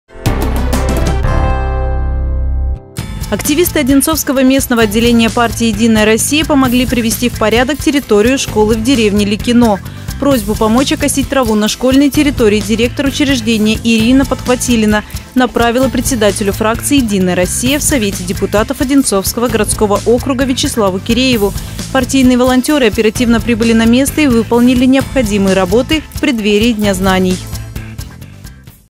Ко Дню знаний
Активисты одинцовского местного отделения партии «Единая Россия» помогли привести в порядок территорию школы в деревне Ликино.